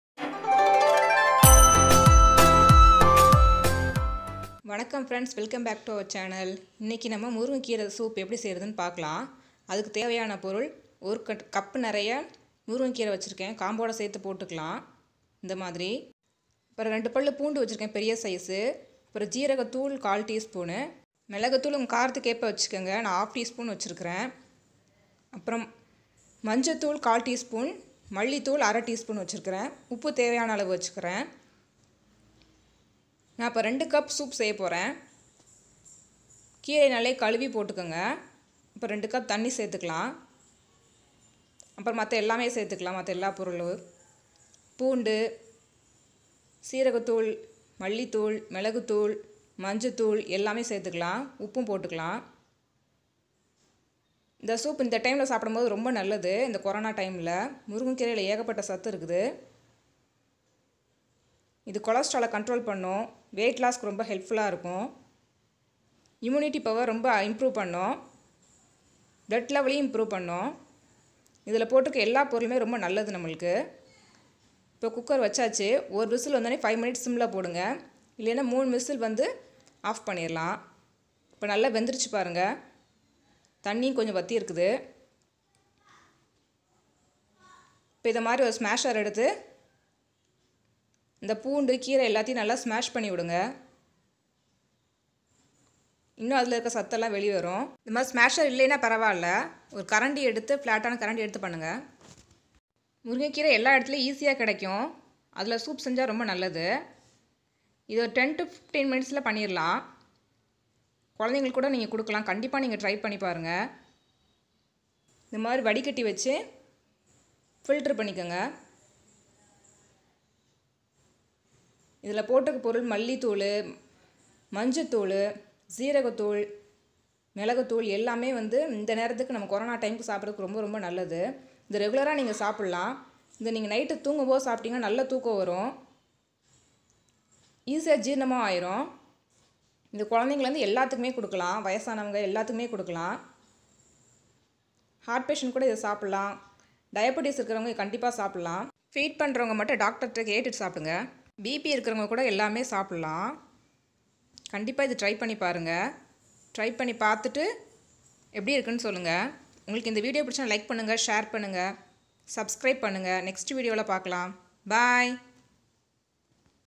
சத்து நிறைந்த முருங்கை கீரை சூப்/ Drumstick leaves soup in Tamil( eng sub)
Healthy and Tasty drumstick leaves soup.

Best soup for this coronna period...

In this vedio, i showed you a very simple and easy soup...

Ingredients:

Drumstick leaves - 1 cup
Garlic - 2 big size

Drumstick leaves are easily available all over the world...

One of the most important health benefits of drumsticks is that it promotes a healthy bone structure due to the presence of high amount of calcium and iron...
The leaves have 7 times more vitamin C than oranges and 15 times more potassium than bananas. It also has calcium, protein, iron, and amino acids, which help your body heal and build muscle. It's also packed with antioxidants, substances that can protect cells from damage and may boost your immune system...

Put the leaves in hot water for 15 minutes and drink it before you go to bed for a good rest at night.

All you need to do is get your hands on some fresh leaves, dehydrate them and then grind them to make a powder...

You can easily make this soup within 10 mins .

If u like this vedio give me a thumbs up👍 ...

And leave a comment on a comment section📝